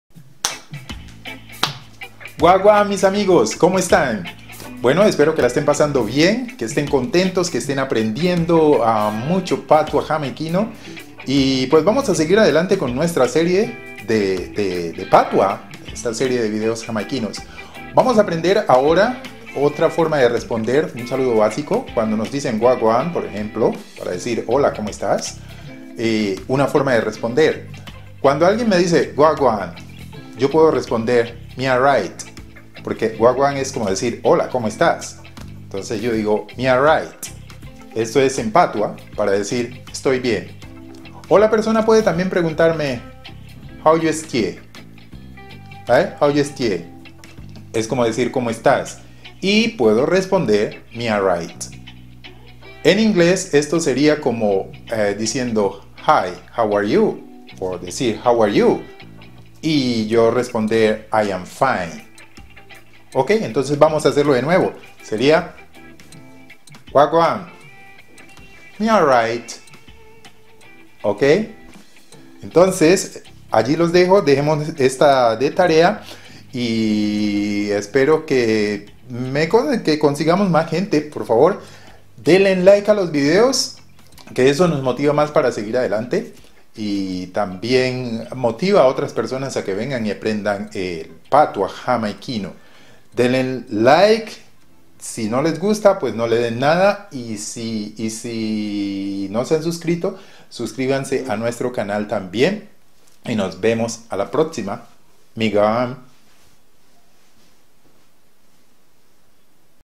Patois Jamaiquino | Mi aright | Walinton Mosquera
Otra forma de responder a un saludo en patua Jamaiquino.